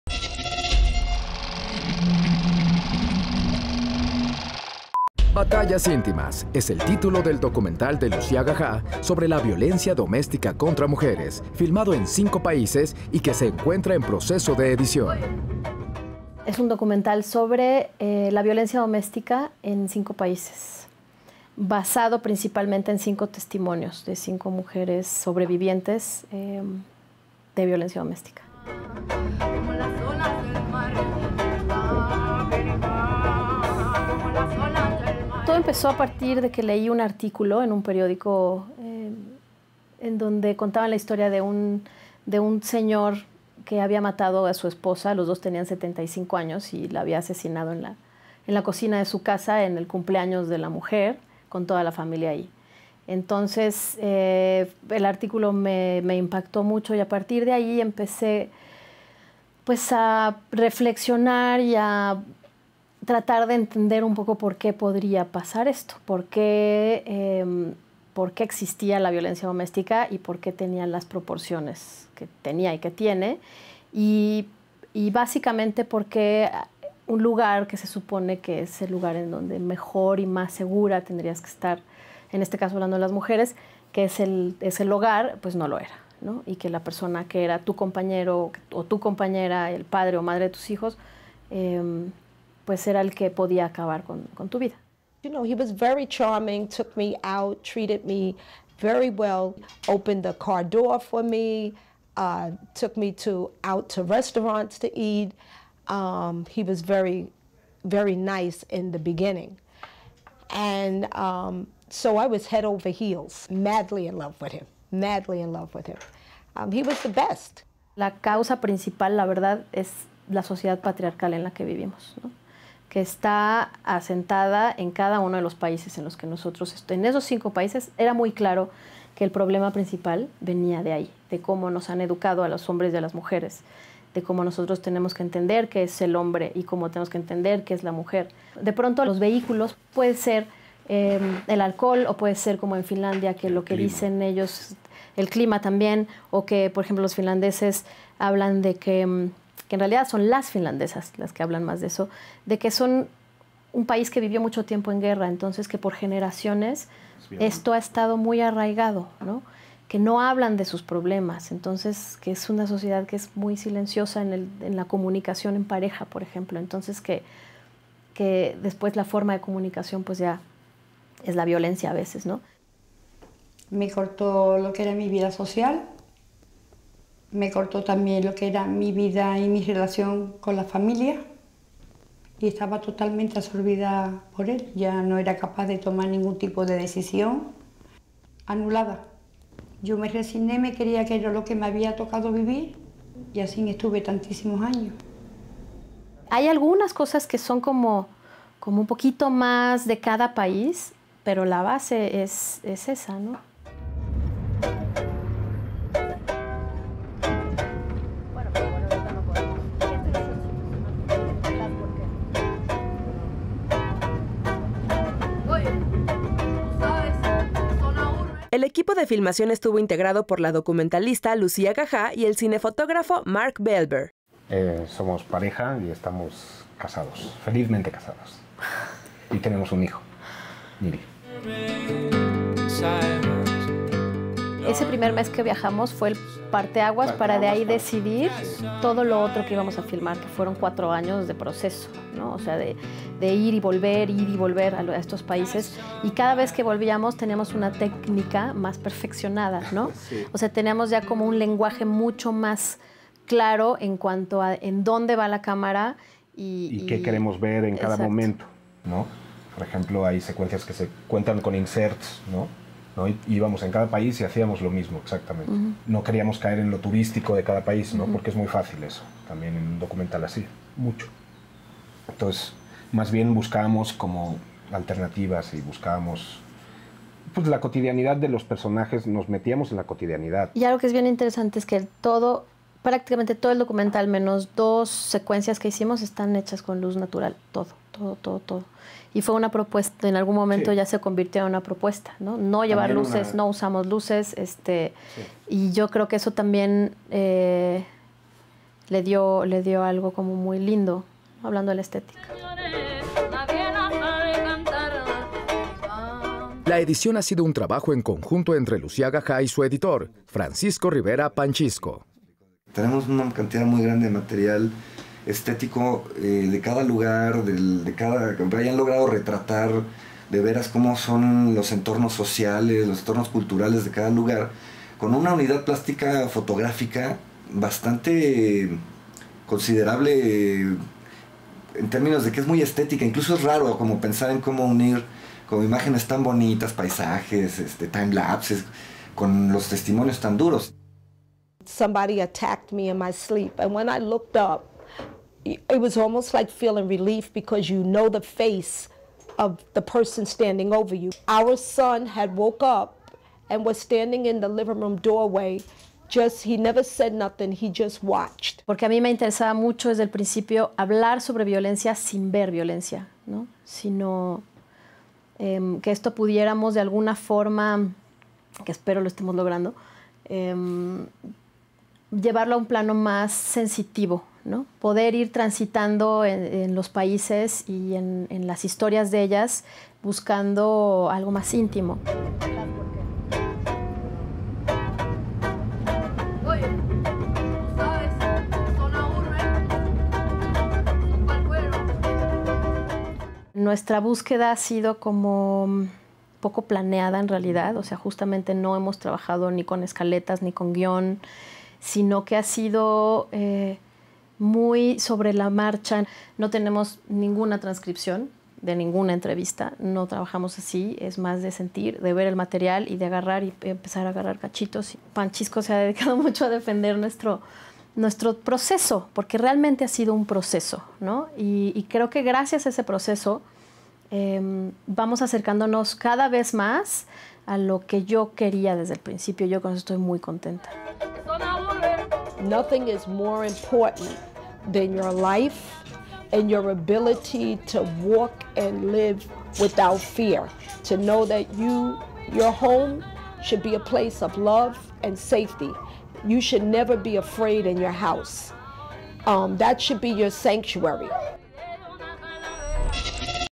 En edición: Batallas íntimas
Entrevista a:

Lucía Gajá, (Directora)
Marc Bellver, (Cinefotógrafo)
Francisco Rivera "Panchisco", (Editor)